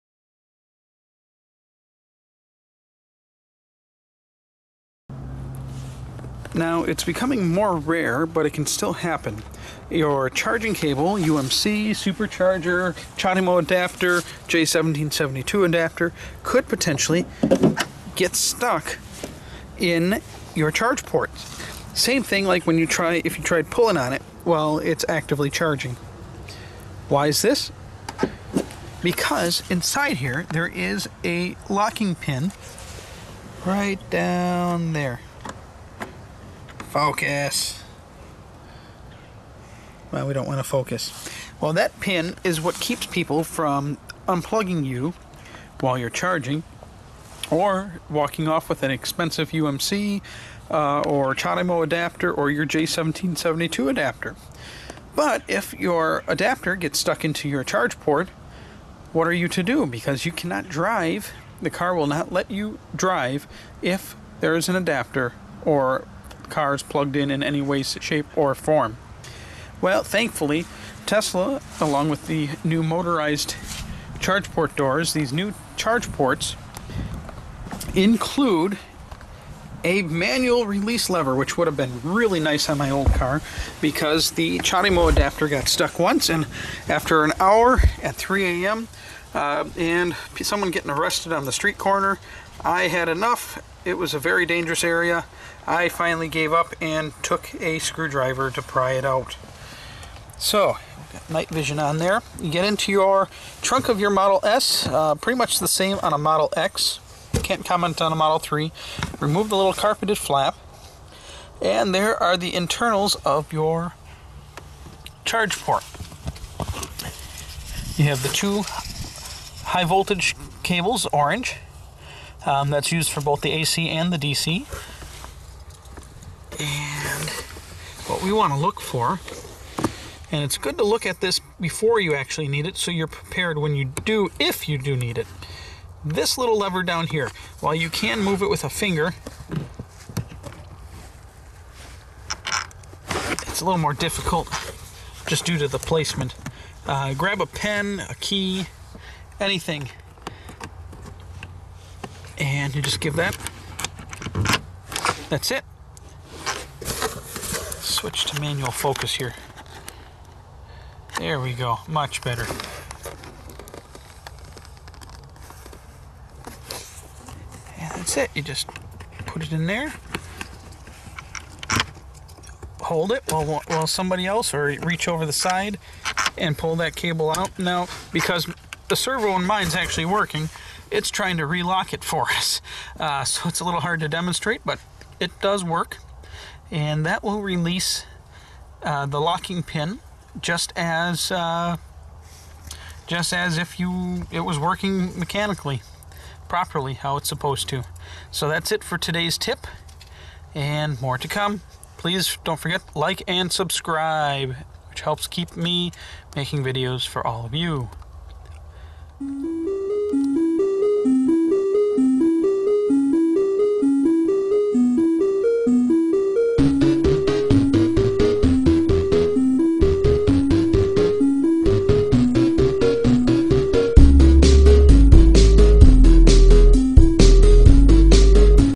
Tesla Emergency Charge Port Release Lever
A just in case type of thing to keep in mind. Will you need this info? Doubt it, but, if/when you do, it's great to know!
   Keep in mind, 2012, 2013 and some 2014 Tesla vehicles do NOT have the emergency release lever!!!